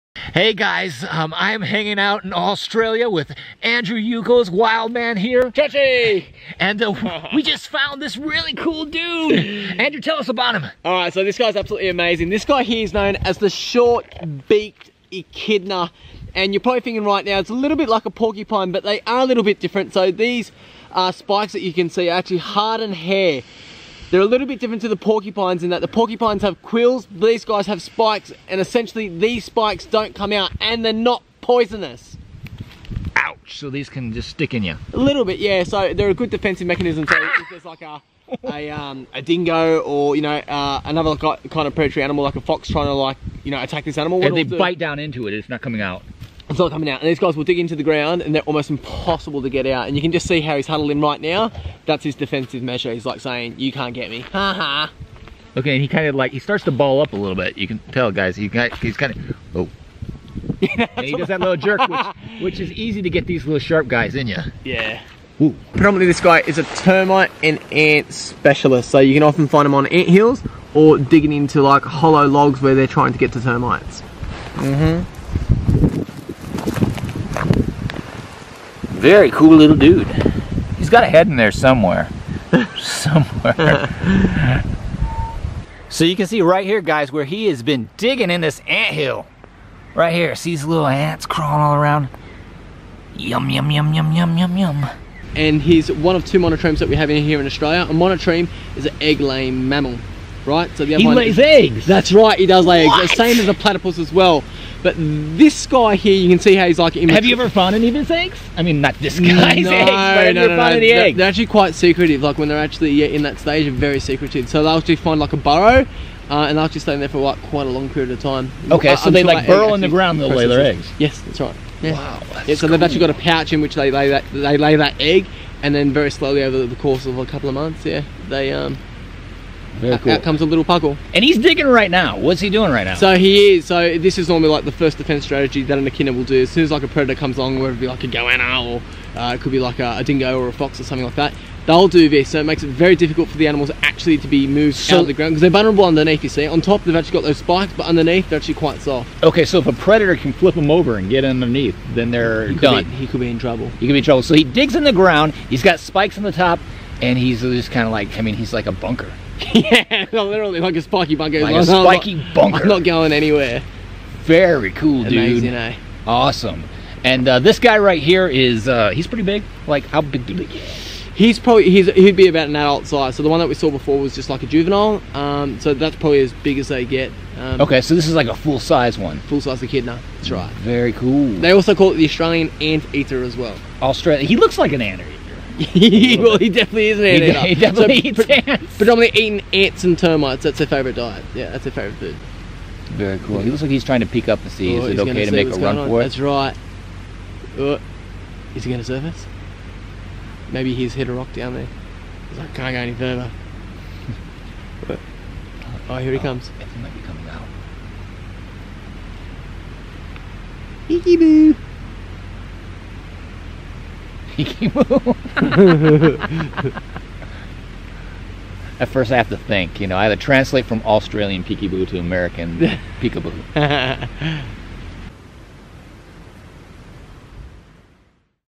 Weird Australian Anteater!
Let's learn about this amazing Australian Anteater called an Echidna with Andrew Ucles!